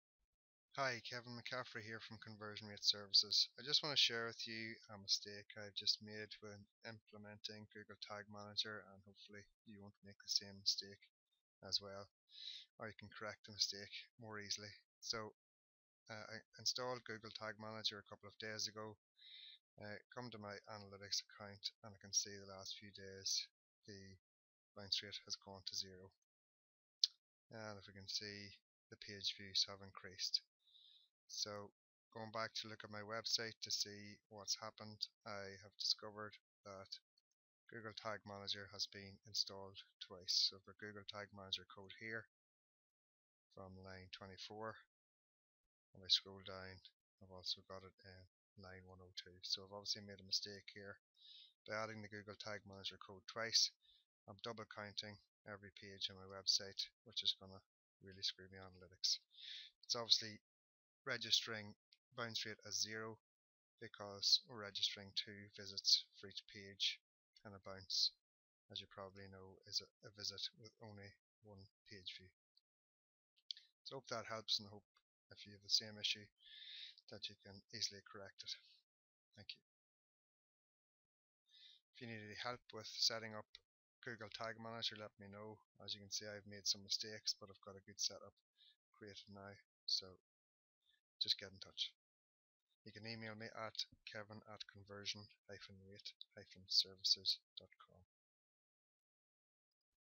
Google Tag Manager Bounce Rate Zero
If you've recently added Google Tag manager and your Google Analytics bounce rate has dropped to zero, this may be one issue causing that.